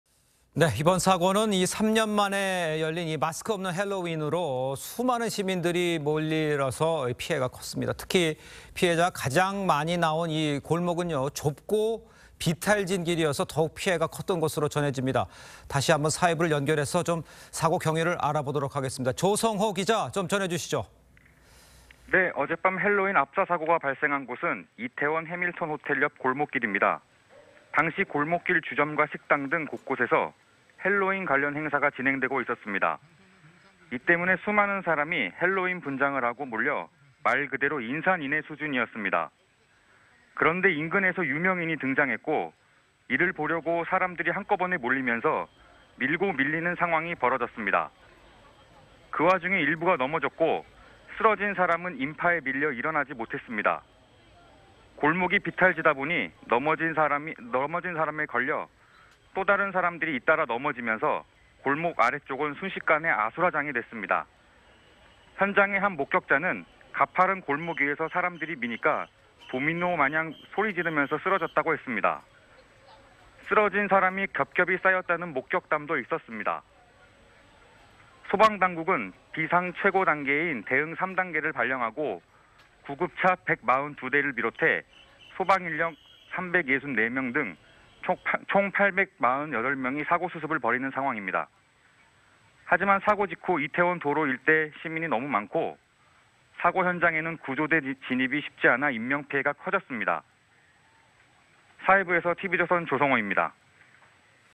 좁은 골목길에서 밀리며 "도미노처럼 쓰러져"
이번 사고는 3년 만의 열린 ‘마스크 없는’ 핼러윈으로 수많은 시민들이 몰린터라 피해가 컸습니다. 특히 피해자가 가장 많이 나온 골목은 좁고 비탈진 길이라 더욱 피해가 컸던 것으로 전해집니다. 다시 사회부 연결합니다.